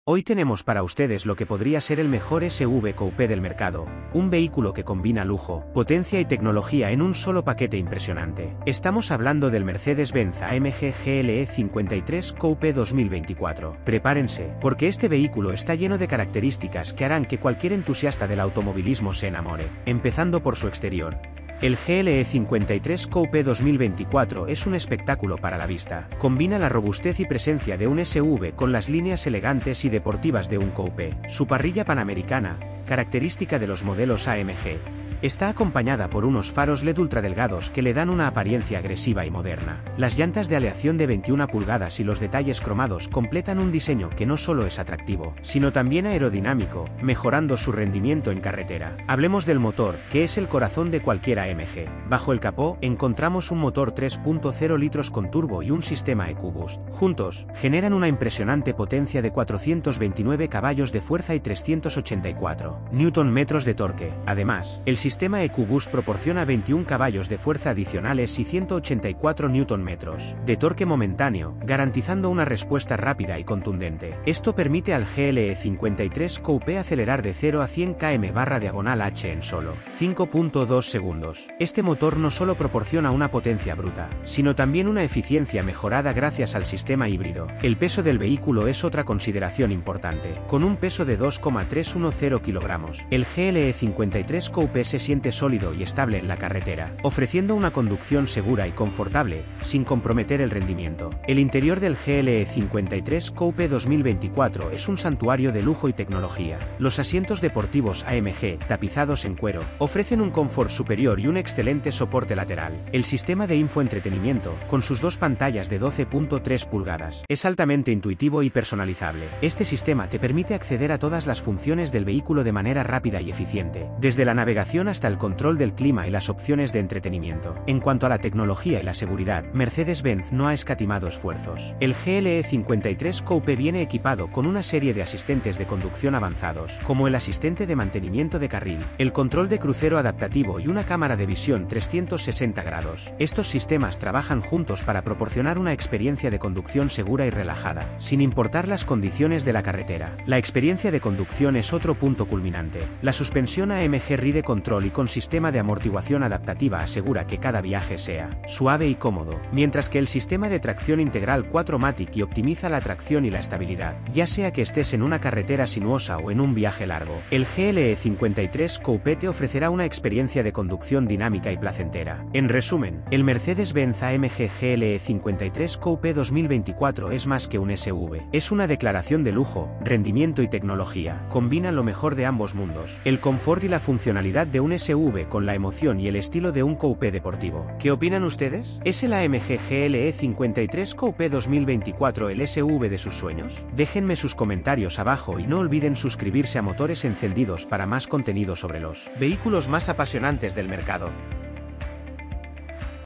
SI TE PREGUNTARAN CUAL ES EL MEJOR SUV ¿QUE DIRIAS? // Mercedes Benz AMG GLE 53 Coupé
¡Bienvenidos a Motores Encendidos! En el video de hoy, exploramos en detalle el impresionante Mercedes-Benz AMG GLE 53 Coupé 2024. Este SUV coupé combina a la perfección el lujo, la potencia y la tecnología más avanzada para ofrecer una experiencia de conducción inigualable.

🔹 Diseño Exterior: Descubre el diseño robusto y aerodinámico con la emblemática parrilla Panamericana y los faros LED ultradelgados que le dan una apariencia agresiva y moderna.

🔹 Potencia y Rendimiento: Con un motor 3.0 litros inline-6 turbo y el sistema EQ Boost, este vehículo genera 429 caballos de fuerza y acelera de 0 a 100 km/h en solo 5.2 segundos. ¡Un verdadero monstruo en la carretera!

🔹 Interior de Lujo: Los asientos deportivos AMG en cuero Nappa, el sistema de infoentretenimiento MBUX con dos pantallas de 12.3 pulgadas y los detalles de alta calidad hacen del interior un lugar cómodo y tecnológico.

🔹 Seguridad y Tecnología: Equipado con asistentes de conducción avanzados como el asistente de mantenimiento de carril y el control de crucero adaptativo, este SUV garantiza una conducción segura y relajada.

🔹 Experiencia de Conducción: La suspensión AMG Ride Control+ y el sistema de tracción integral 4MATIC+ aseguran una experiencia de conducción suave y estable en cualquier condición.

¡Dejen sus comentarios y dígannos qué opinan del AMG GLE 53 Coupé 2024!